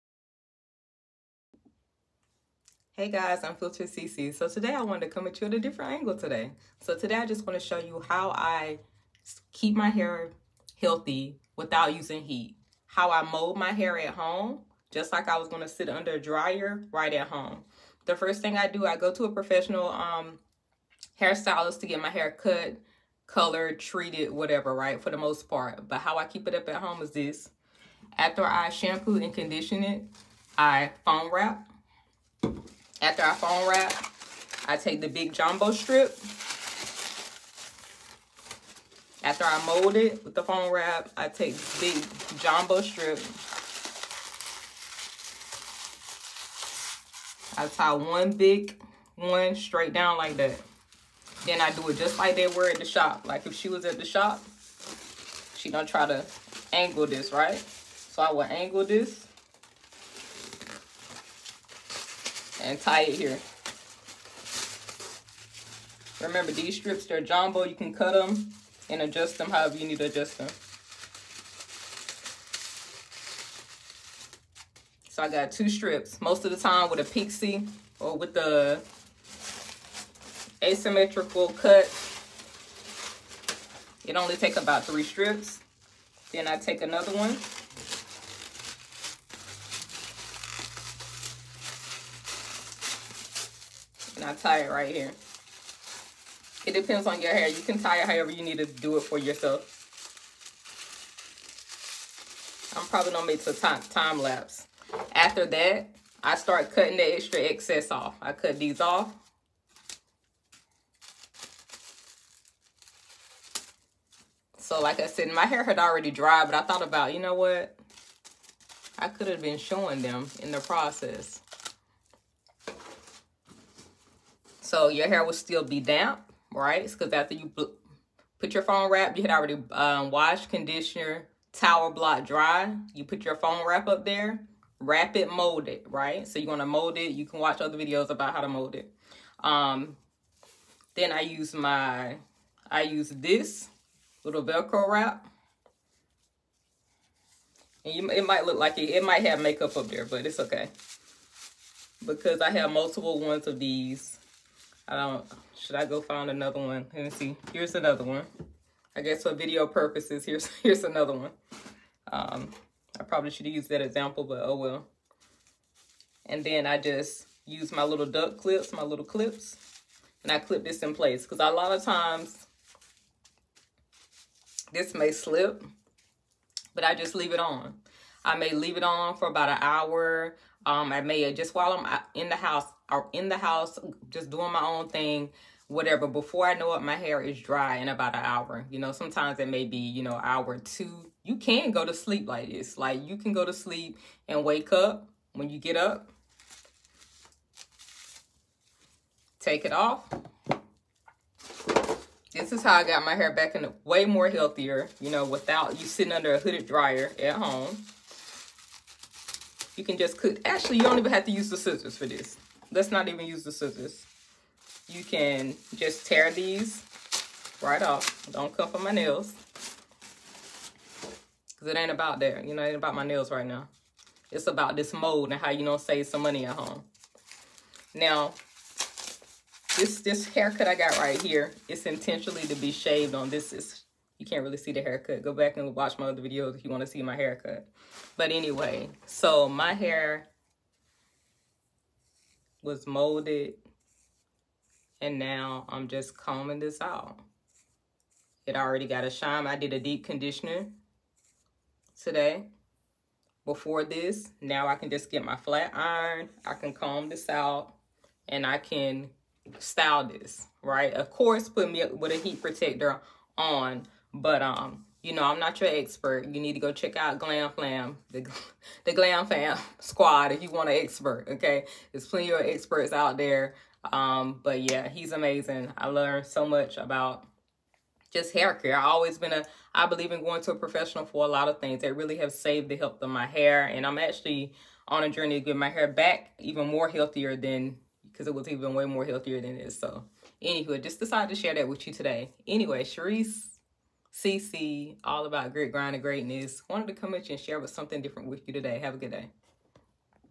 HOW I WRAP/ MOLD MY HAIR NO DRYER OPTION
How I save money on Hair Care, The Key is Go to a Professional for Haircuts and certain treatments for Example Hair Color, relaxers, and even some liquid proteins. Knowing when and where to spend the money .